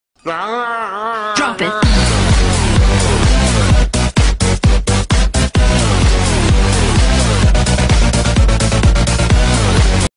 college green screen test (Nigel Thornberry edition)
ok well today in college our lecturer gave us a video clip filmed on a green screen and we had to give it a back ground so i thought i would have a little fun and add a bit extra, this is my result